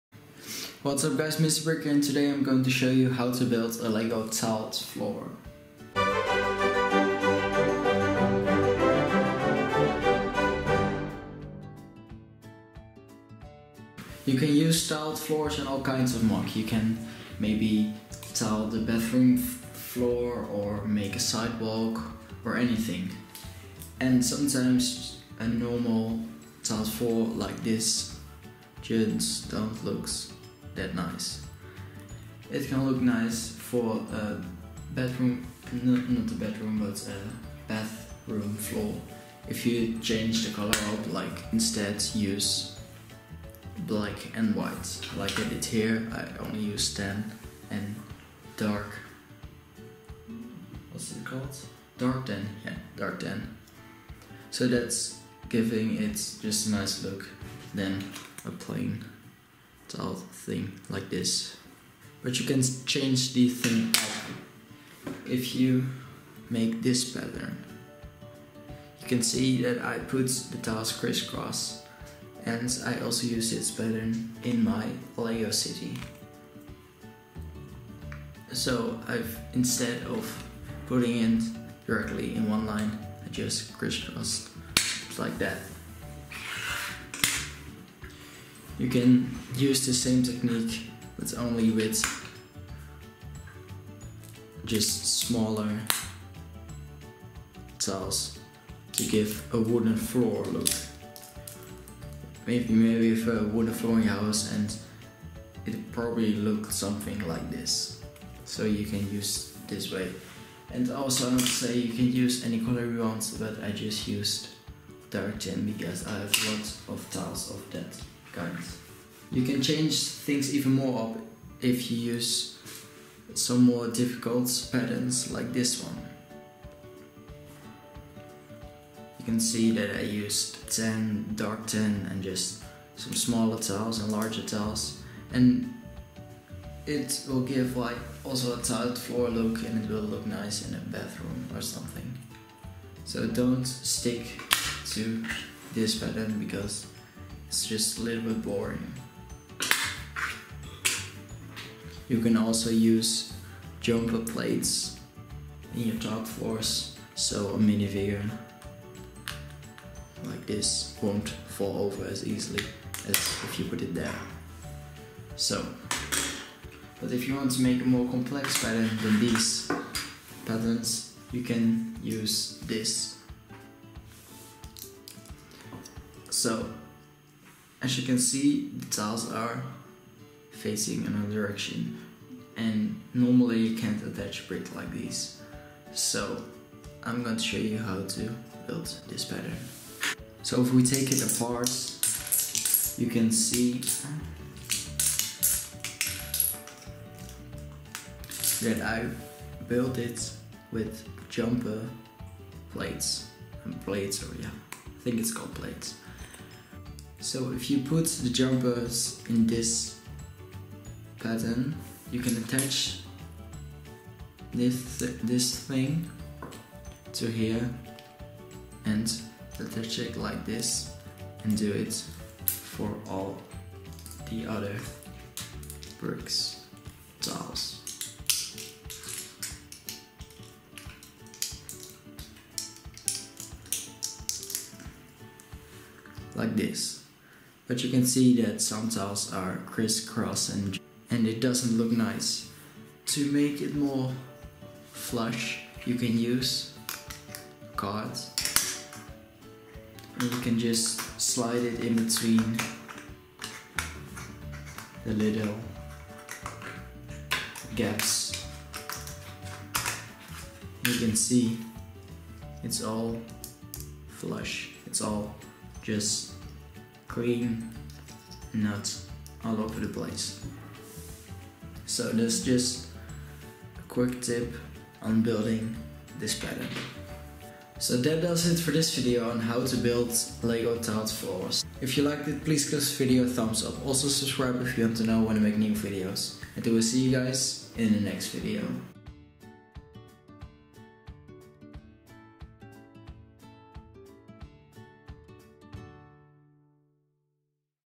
How do you build #3 | Tiled floors
Today I'm bringing you another How do you build. This time I'm going to show you how to build lego tiled floors!!!